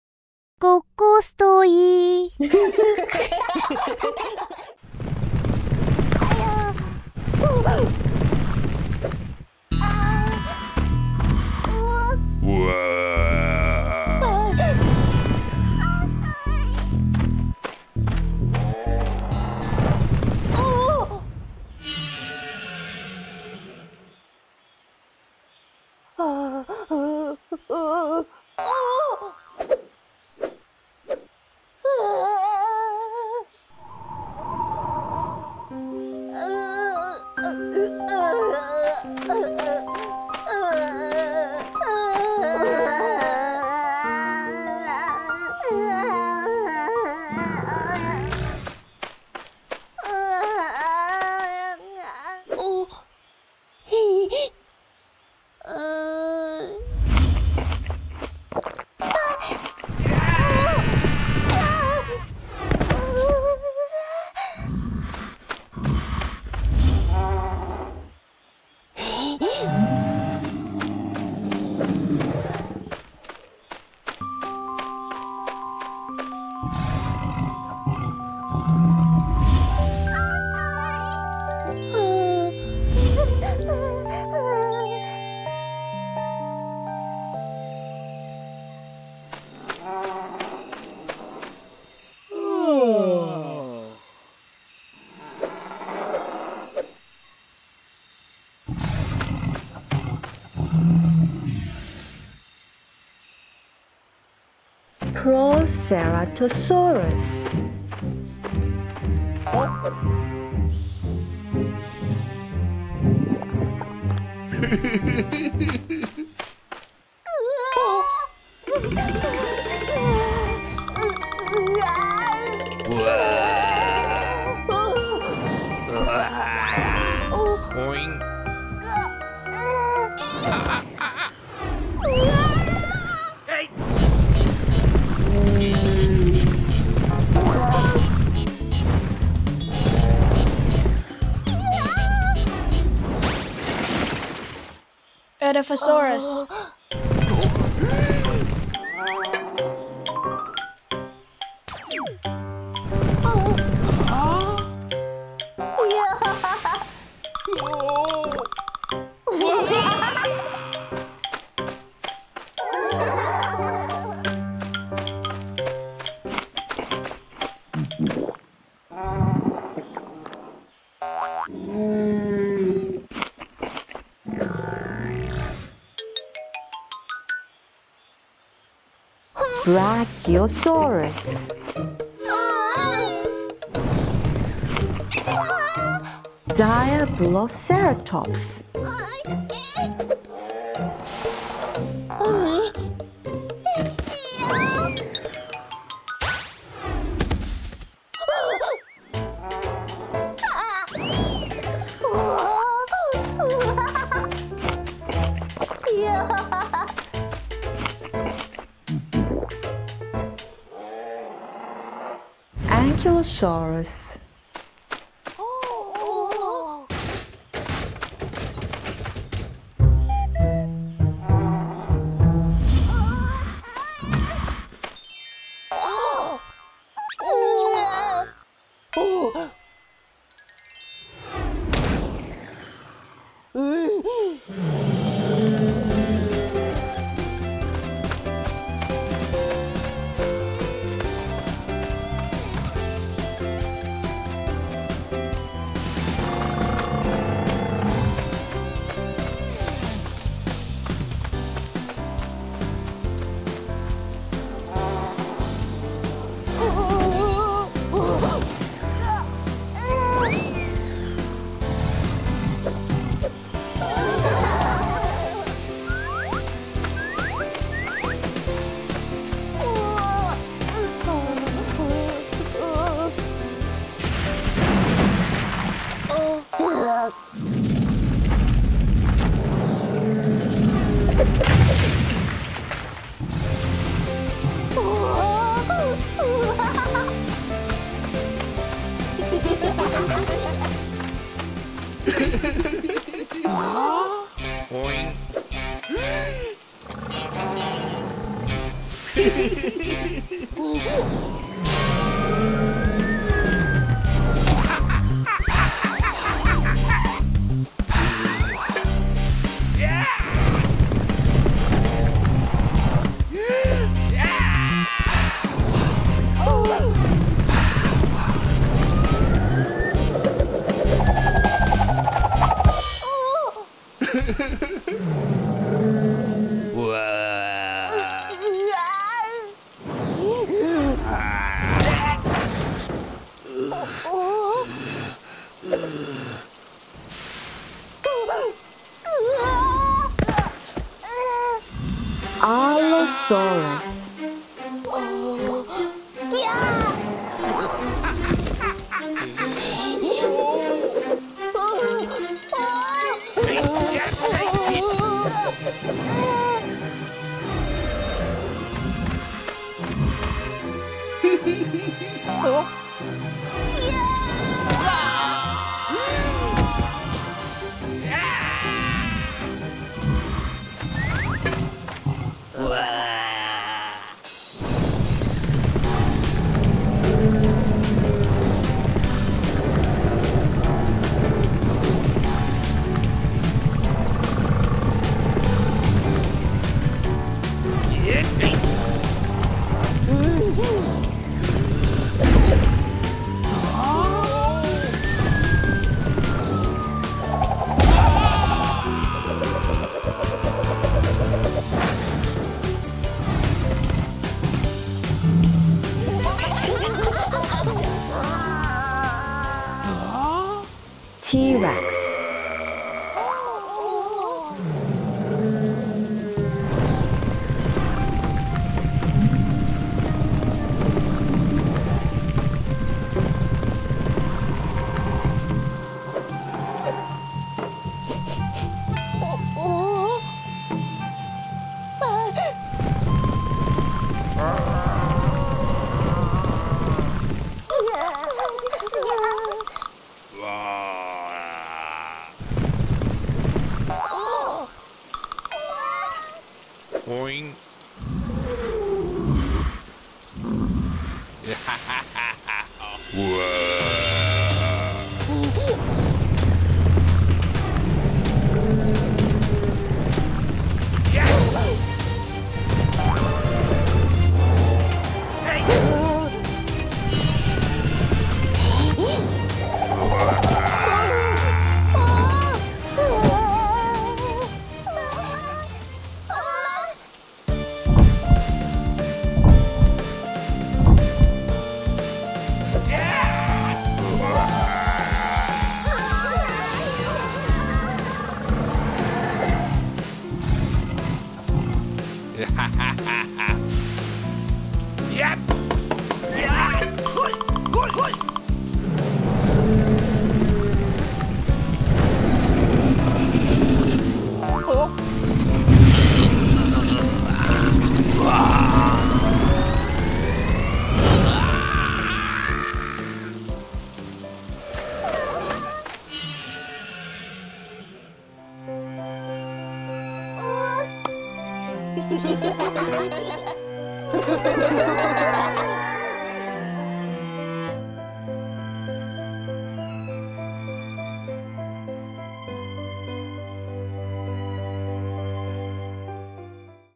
조금만 기다려, 우리가 달려갈게!ㅣ공룡싸움, 초식공룡ㅣ꼬꼬스토이
오늘 소개해드릴 공룡은 에다포사우루스, 디아블로케라톱스, 안킬로사우루스, 알로사우루스, 티라노사우루스, 프로케라토사우루스, 브라키오사우루스입니다.
꼬꼬스토이와 함께 즐거운 하루 보내세요!